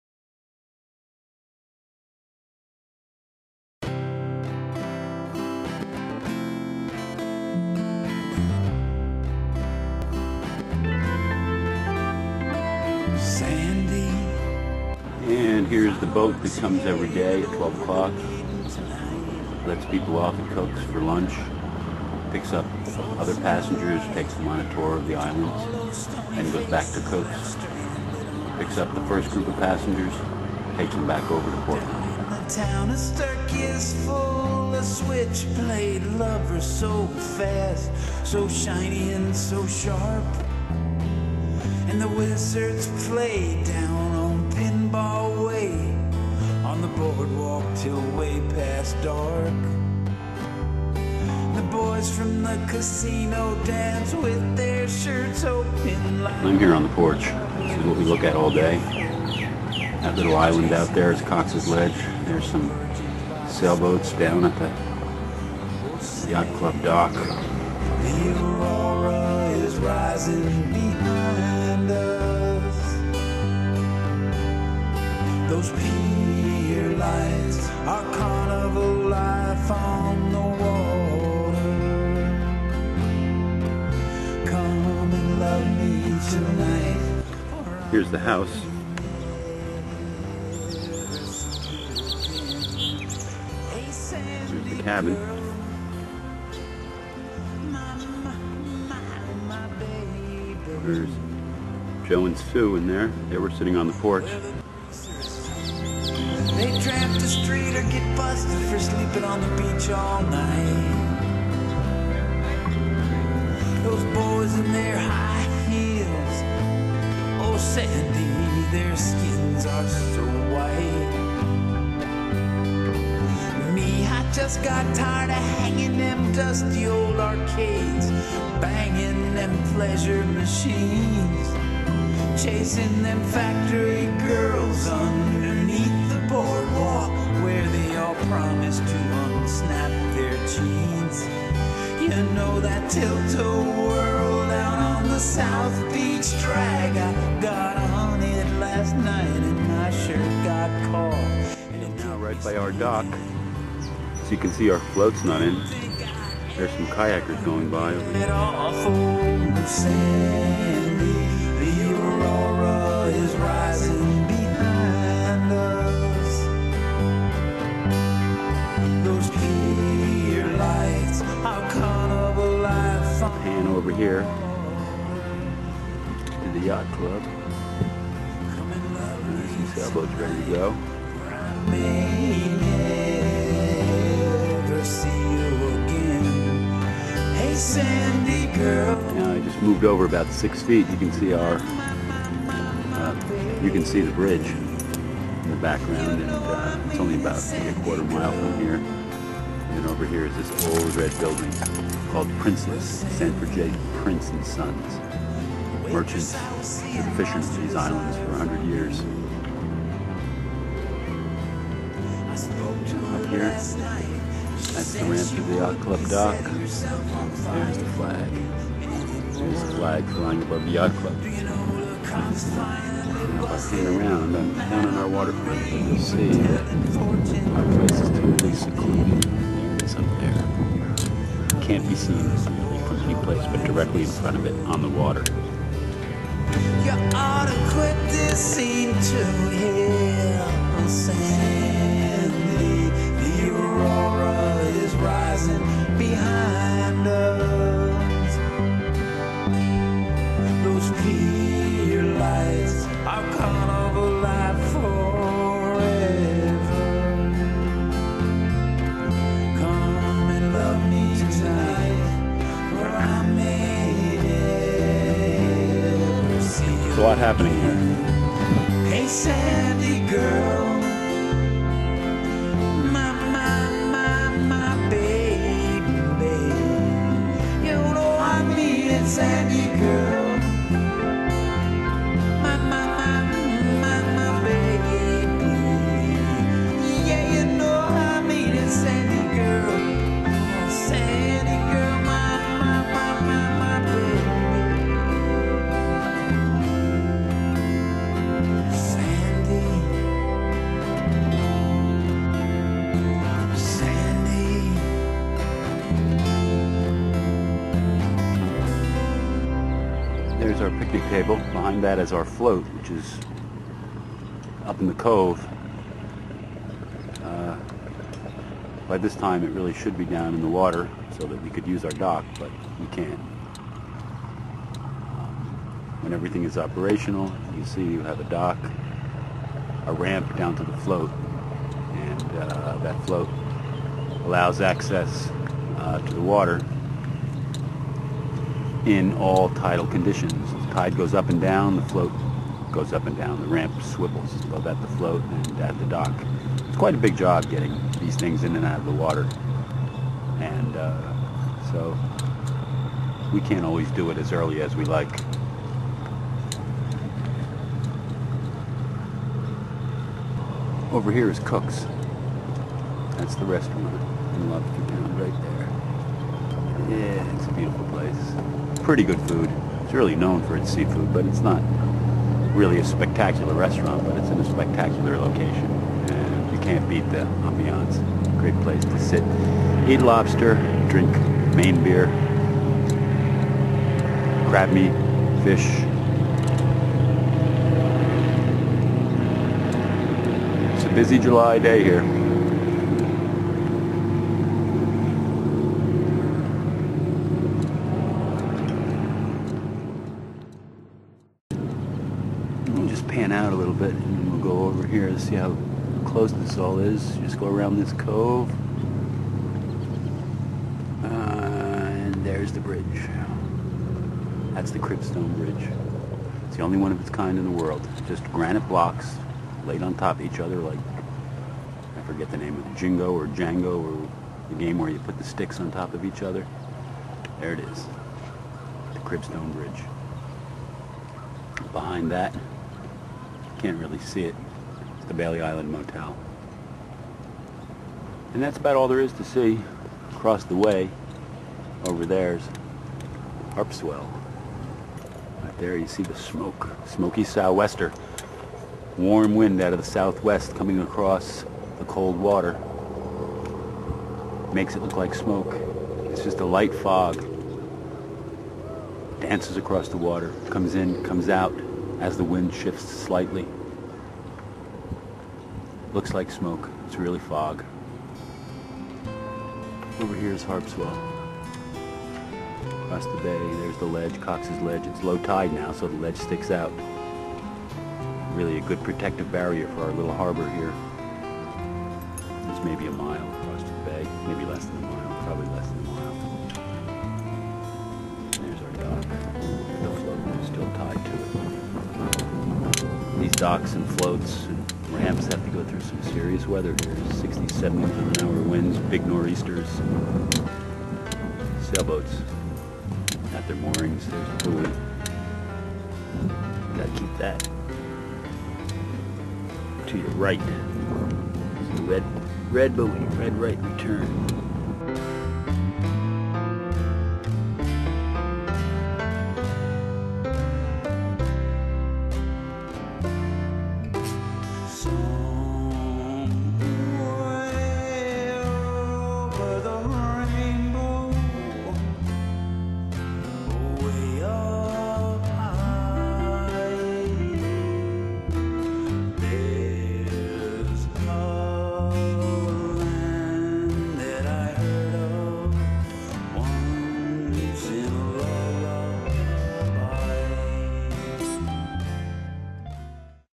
Orr's Island
A July day on Maine's mid coast